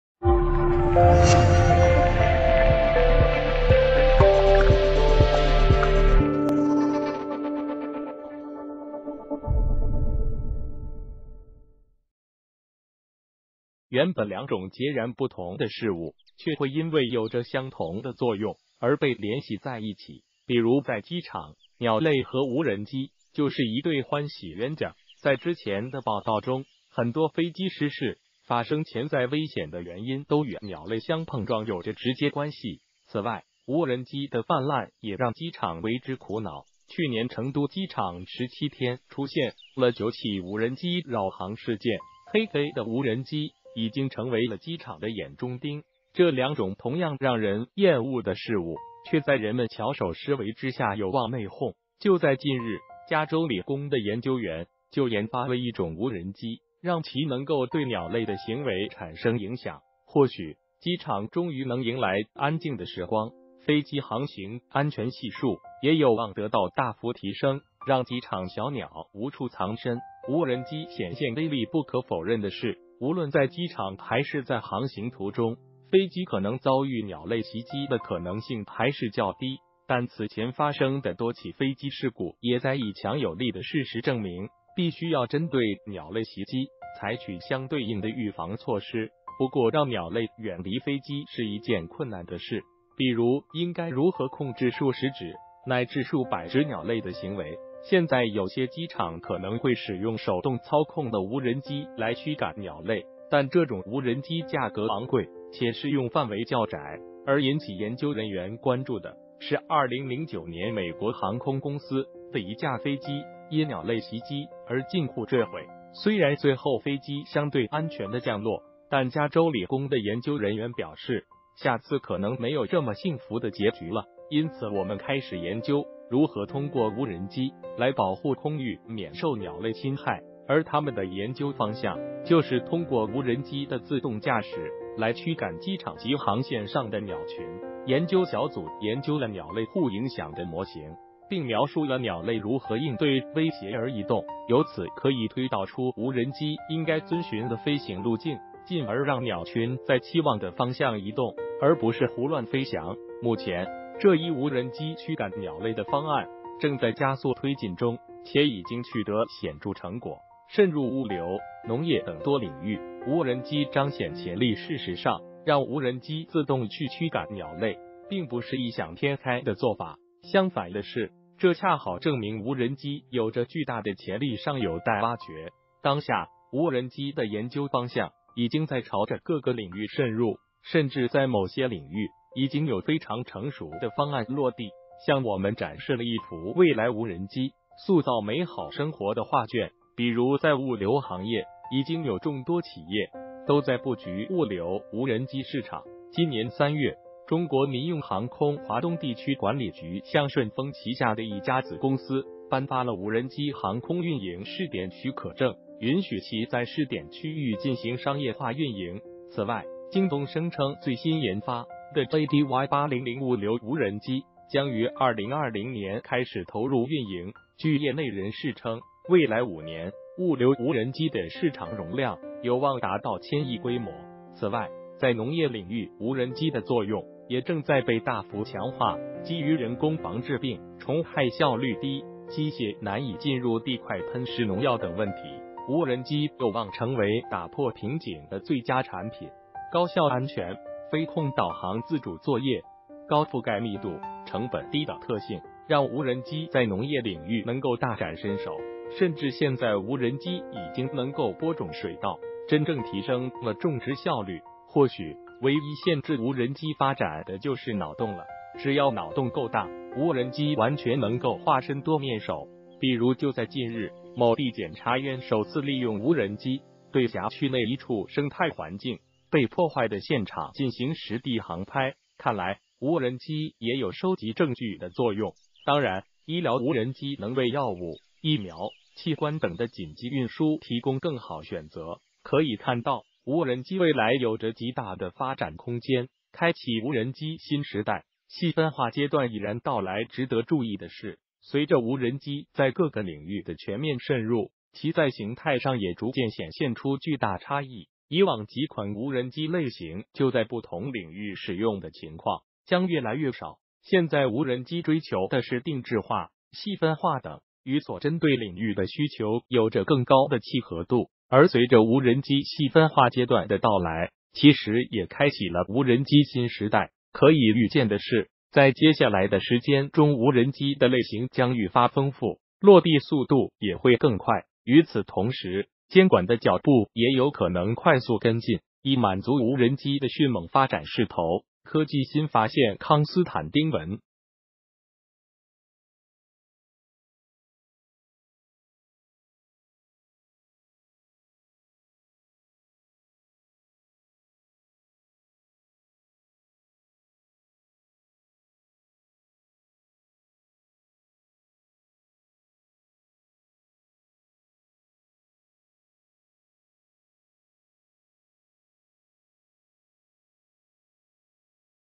干掉机场的小鸟！无人机的未来比你想象的更光明
谢谢！原本两种截然不同的事物，却会因为有着相同的“作用”而被联系在一起。比如在机场，鸟类和无人机就是一对“欢喜冤家”。在之前的报道中，很多飞机失事、发生潜在危险的原因都与鸟类相碰撞有着直接关系。此外，无人机的泛滥也让机场为之苦恼。去年成都机场17天出现了9起无人机扰航事件，“黑飞”的无人机已经成为了机场的“眼中钉”。这两种同样让人“厌恶”的事物，却在人们巧手施为之下有望“内讧”。就在近日，加州理工的研究员就研发了一种无人机，让其能够对鸟类的行为产生影响。或许，机场终于能迎来“安静”的时光，飞机航行安全系数也有望得到大幅提升。让机场小鸟无处藏身！无人机显现威力不可否认的是，无论在机场还是在航行途中，飞机可能遭遇鸟类袭击的可能性还是较低。但此前发生的多起飞机事故也在以强有力的事实证明，必须要针对鸟类袭击采取相对应的预防措施。不过让鸟类远离飞机是一件困难的事，比如应该如何控制数十只乃至数百只鸟类的行为？现在有些机场可能会使用手动操控的无人机来驱赶鸟类，但这种无人机价格昂贵，且适用范围较窄。而引起研究人员关注的，是2009年美国航空公司的一架飞机因鸟类袭击而近乎坠毁。虽然最后飞机相对安全地降落，但加州理工的研究人员表示，“下次可能没有这么幸福的结局了，因此我们开始研究如何通过无人机来保护空域免受鸟类侵害。”而他们的研究方向，就是通过无人机的自动驾驶来驱赶机场及航线上的鸟群。研究小组研究了鸟类互影响的模型，并描述了鸟类如何应对威胁而移动。由此可以推导出无人机应该遵循的飞行路径，进而让鸟群在期望的方向移动而不是胡乱飞翔。目前，这一无人机驱赶鸟类的方案正在加速推进中，且已经取得显著成果。渗入物流、农业等多领域，无人机彰显潜力事实上，让无人机自动去驱赶鸟类并不是异想天开的做法。相反的是，这恰好证明无人机有着巨大的潜力尚有待挖掘。当下，无人机的研究方向已经在朝着各个领域渗入。甚至在某些领域，已经有非常成熟的方案落地，向我们展示了一幅未来无人机塑造美好生活的画卷。比如在物流行业，已经有众多企业都在布局物流无人机市场。今年3月，中国民用航空华东地区管理局向顺丰旗下的一家子公司颁发了“无人机航空运营（试点）许可证”，允许其在试点区域进行商业化运营。此外，京东声称最新研发的JDY-800物流无人机将于2020年开始投入运营。据业内人士称，未来5年，物流无人机的市场容量有望达到千亿规模。此外，在农业领域无人机的作用也正在被大幅强化。基于人工防治病虫害效率低、机械难以进入地块喷施农药等问题，无人机有望成为打破瓶颈的最佳产品。高效安全、飞控导航自主作业、高覆盖密度、成本低等特性，让无人机在农业领域能够大展身手。甚至现在无人机已经能够播种水稻，真正提升了种植效率。或许，唯一限制无人机发展的就是“脑洞”了。只要脑洞够大，无人机完全能够化身多面手。比如就在近日，某地检察院首次利用无人机对辖区内一处生态环境被破坏的现场进行实地航拍。看来，无人机也有收集证据的作用。当然，医疗无人机能为药物、疫苗、器官等的紧急运输提供更好选择。可以看到，无人机未来有着极大的发展空间。开启无人机新时代，细分化阶段已然到来值得注意的是，随着无人机在各个领域的全面渗入，其在形态上也逐渐显现出巨大差异。以往几款无人机类型就在不同领域使用的情况，将越来越少。现在无人机追求的是定制化、细分化等，与所针对领域的需求有着更高的契合度。而随着无人机细分化阶段的到来，其实也开启了无人机新时代。可以预见的是，在接下来的时间中无人机的类型将愈发丰富，落地速度也会更快。与此同时，监管的脚步也有可能快速跟进，以满足无人机的迅猛发展势头。（科技新发现康斯坦丁/文）